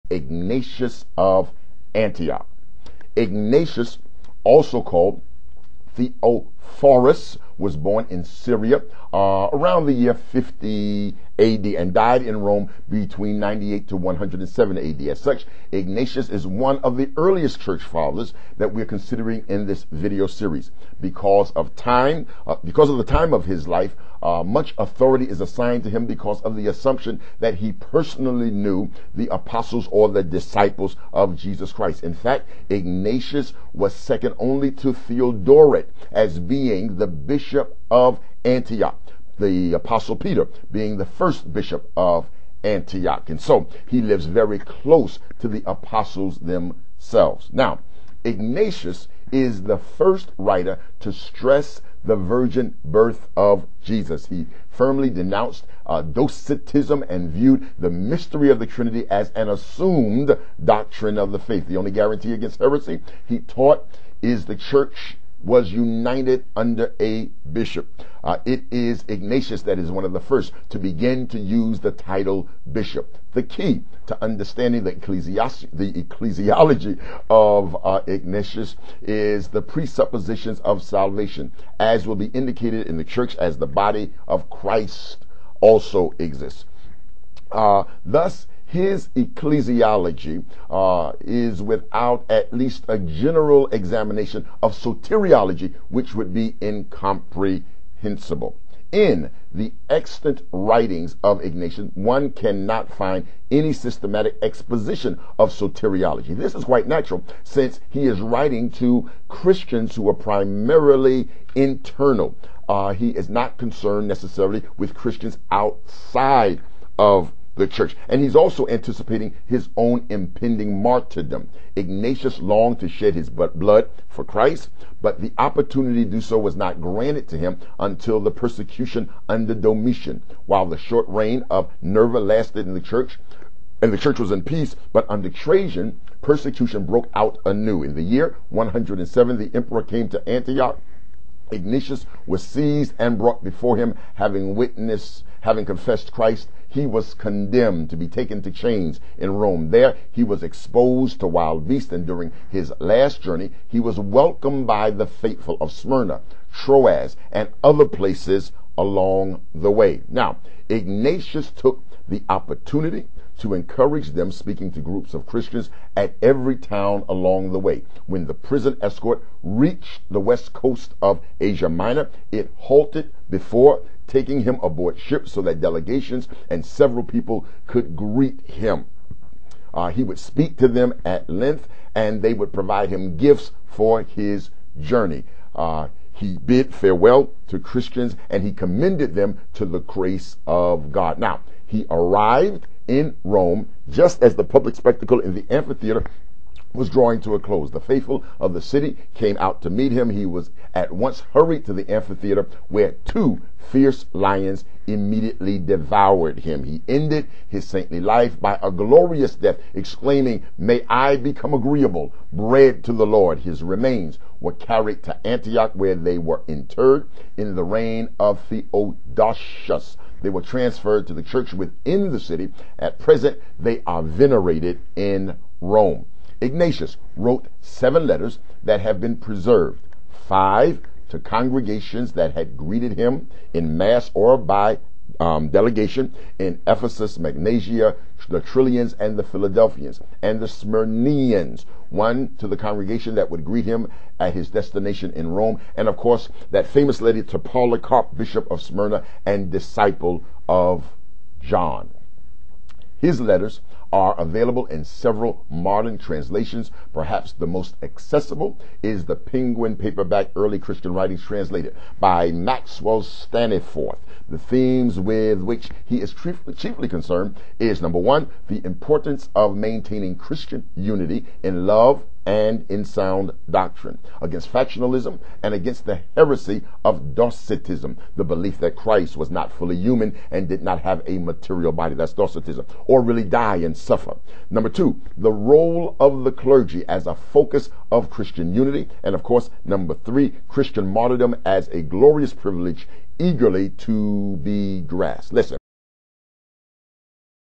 Historical Profile: Ignatius of Antioch
This video examines the life of Ignatius one of the outstanding church leaders of the early church era.  This is part of Bishop Lewter's series on historical figures in the church